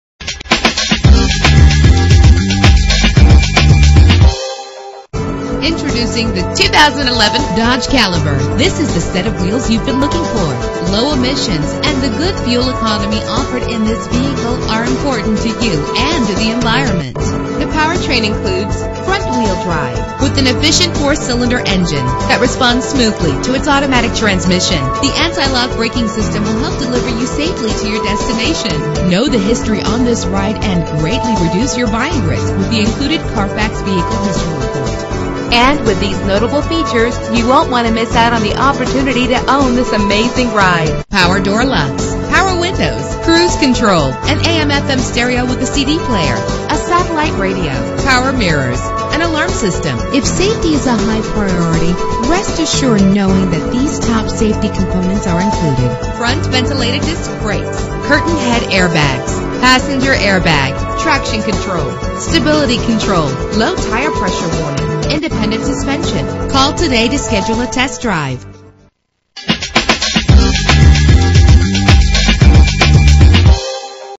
2011 Dodge Caliber - Little Falls NJ
Year: 2011
Make: Dodge
Model: Caliber
Trim: Heat
Engine: 2.0 liter inline 4 cylinder 16 valve
Transmission: Automatic
Color: Bright Silver Clearcoat Metallic
Mileage: 38049
Address: